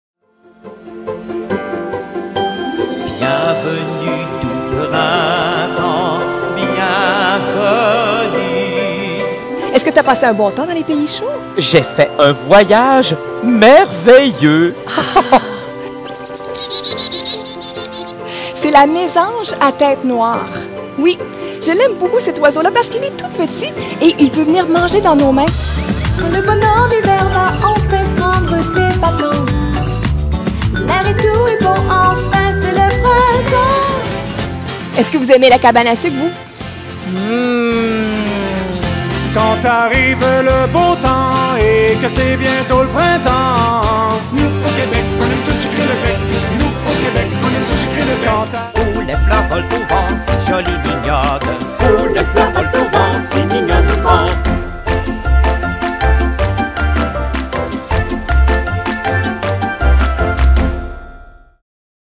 Spectacle de Pâques pour enfants à télécharger
Spectacle interactif à télécharger pour vos cocos à l'occasion de Pâques. Des chansons, des jeux, des apprentissages, de la magie et beaucoup de plaisir!